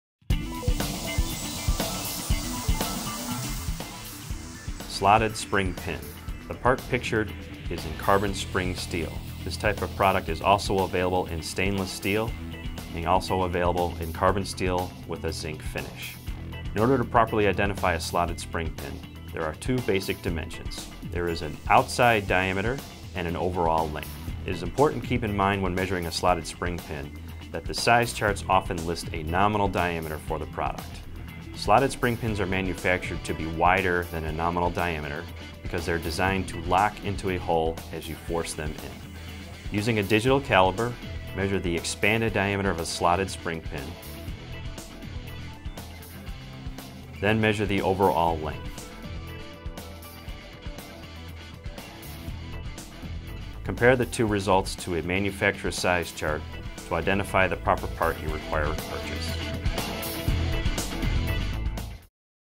How to Identify Slotted Spring Pins
Learn how to measure and identify Driv-Lok SLTP2501000BT and other standard slotted spring pins.